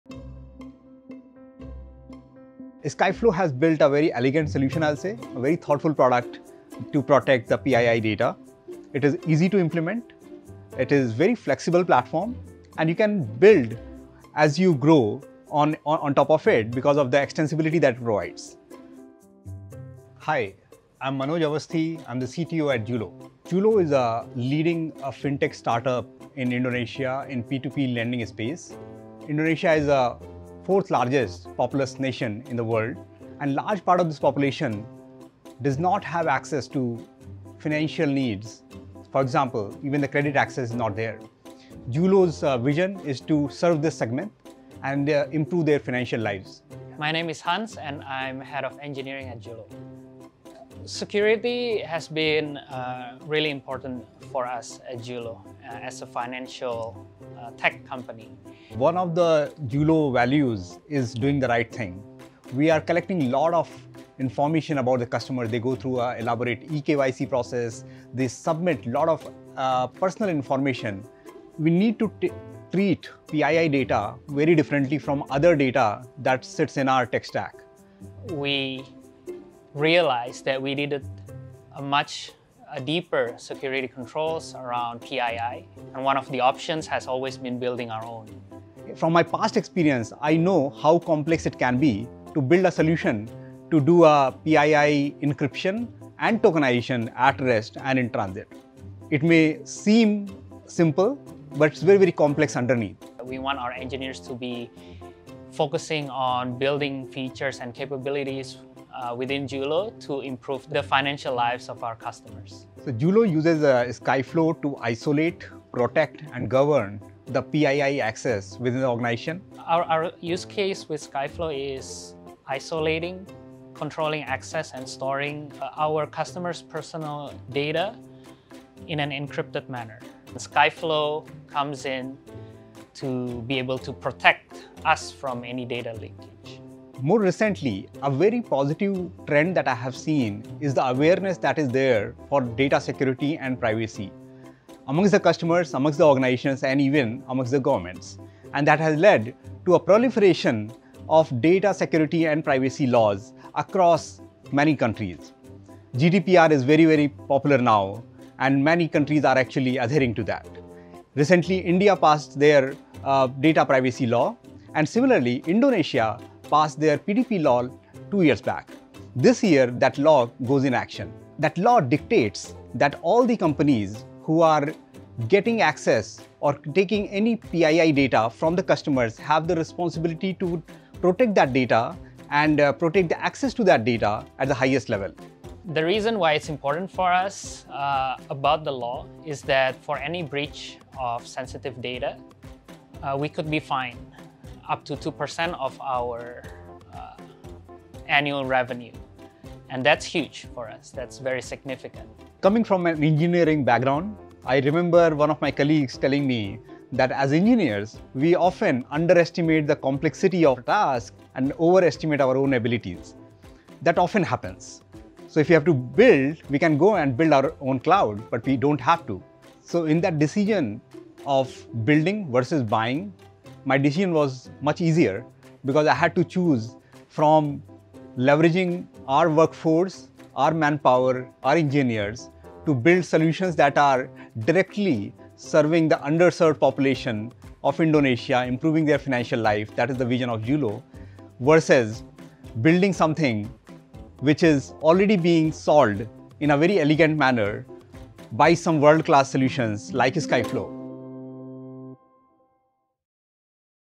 Julo | Skyflow - How Indonesia's Leading Fintech Simplified PII Protection to Meet PDP Compliance
Julo is a leading Indonesian digital lending company that provides affordable, unsecured personal loans for consumers.

Privacy and security is of utmost importance to Julo. In this video, Manoj Awasthi, CTO of Julo, and Hans Sebastian, Head of Engineering of Julo explain why they chose Skyflow to make sure all customer data is secure.